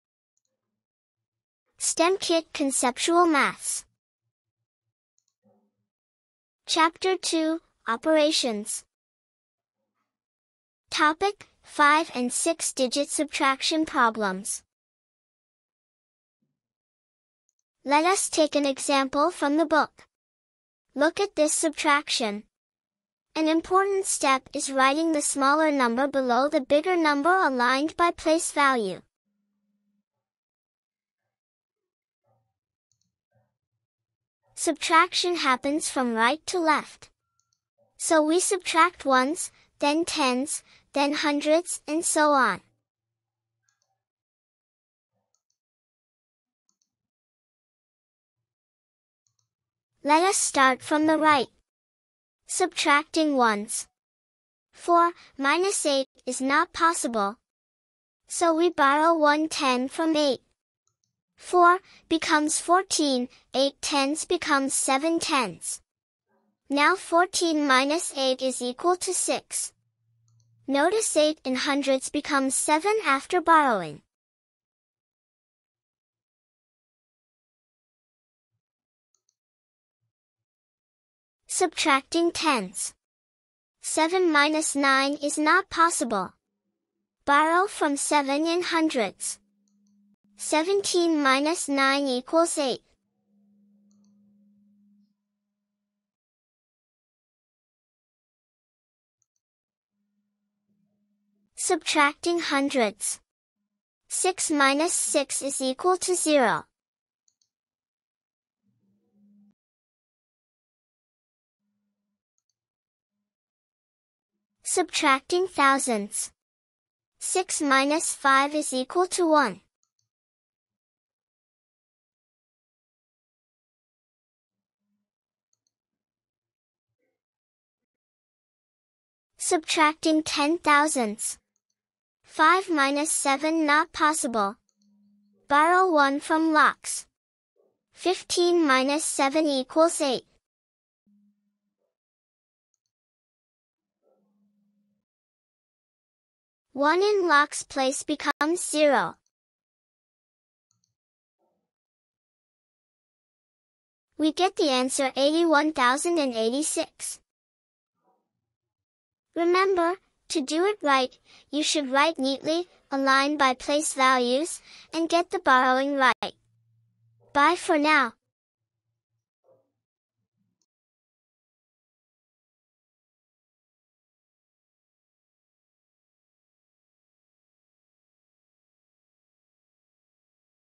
Mastering Subtractions: Borrowing for 5 and 6-Digit Numbers! Solve subtractions faster!
Subtractions with carry over can be daunting for the initiators. Let us see with an example to understand how subtractions are done. Animations make it very easy to understand.

G4 2 6 Operations Subtraction